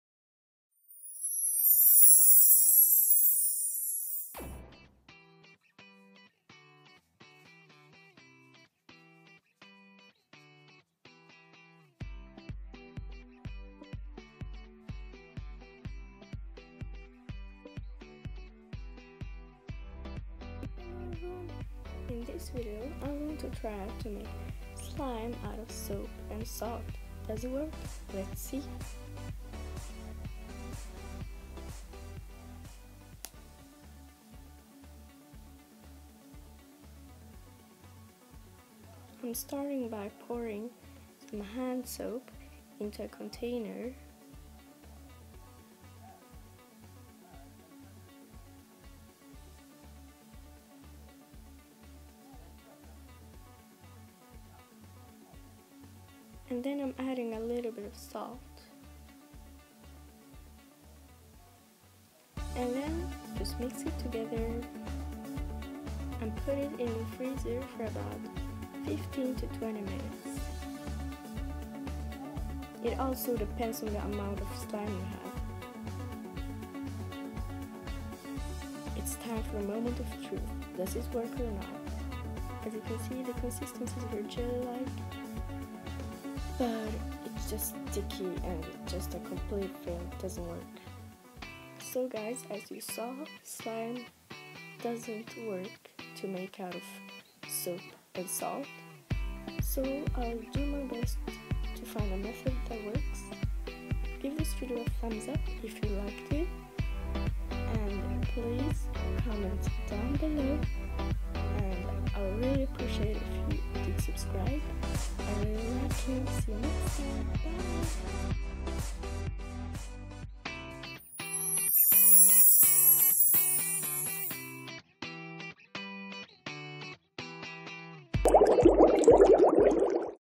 I'm testing: DIY Salt & Soap Slime| Does It Really Work?
Hi Hello! Evryone 🤗  Make your video request in the comments down below👇                                      Instagram: diyloverr2                                                                                                                                                                             Snapchat:  diyloverr2                                                                                                                                                                                   It Dosen't Work To Make Slime Out Of Salt And Soap! But I'll Do My Best To Find A Method That Works.  Bye😄👋